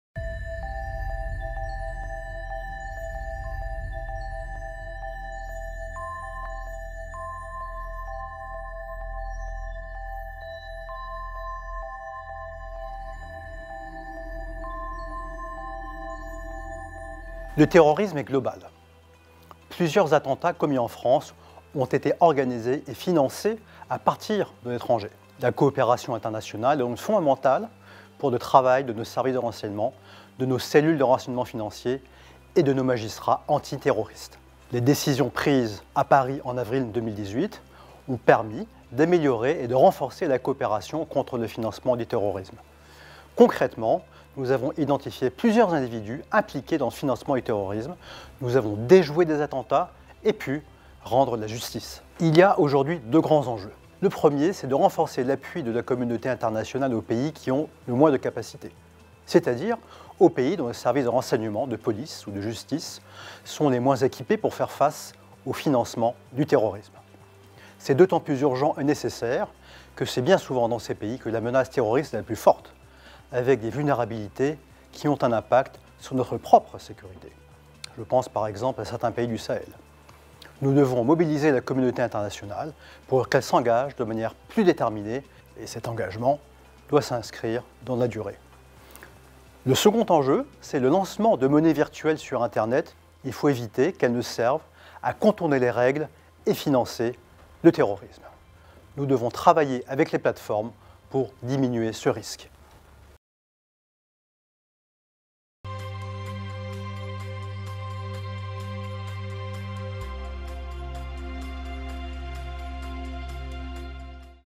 No Money For Terror - 2ème conférence internationale de lutte contre le financement du terrorisme
Le ministre participera à Melbourne à la deuxième conférence internationale de lutte contre le financement du terrorisme « No Money For Terror », dont la première édition s’était tenue à Paris l’an dernier. Cet événement, qui rassemblera 55 pays ainsi qu’une vingtaine d’organisations internationales et régionales, permettra de renforcer la mobilisation internationale pour lutter contre le financement des groupes terroristes.

Interview d'Olivier Caron, Envoyé spécial pour la Stratégie Internationale en matière de Lutte contre le Terrorisme.

Instragram : @francediplo